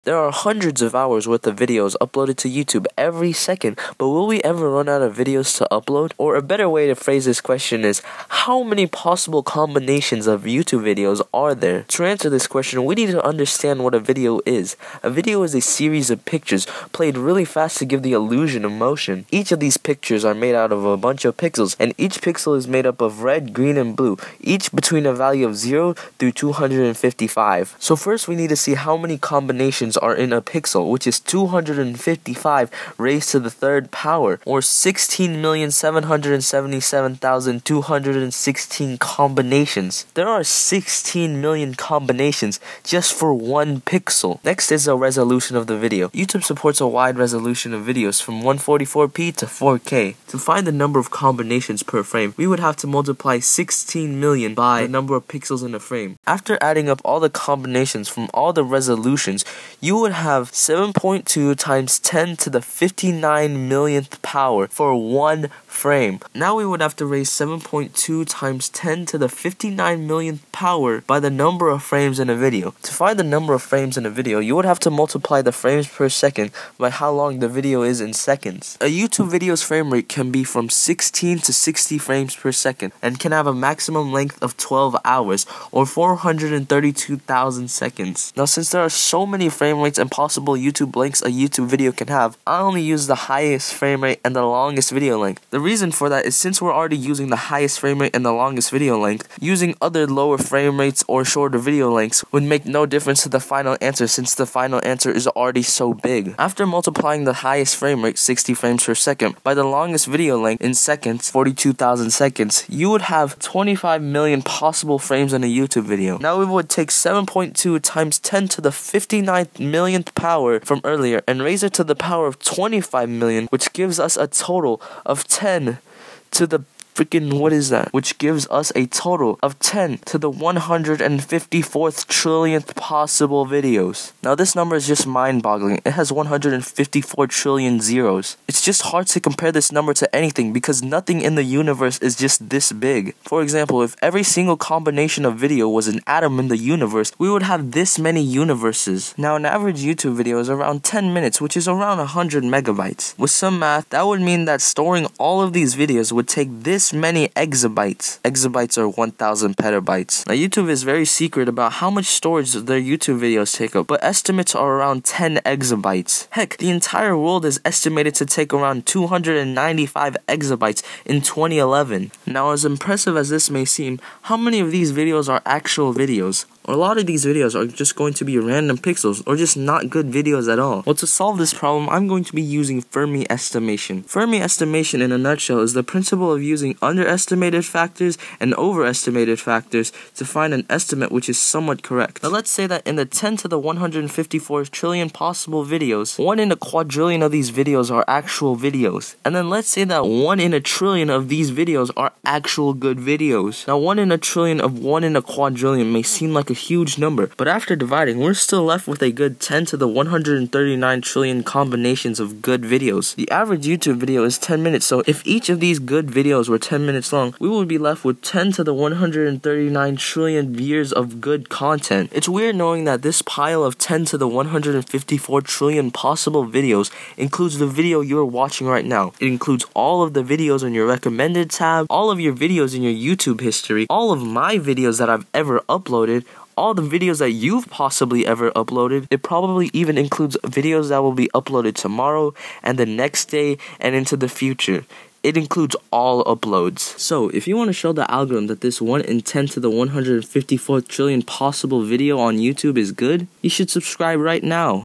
How many Possible YouTube videos are there?
there are probably trillions of youtube videos out there, but i wanted to know definitively how many possible youtube videos can be made, so i answered that question.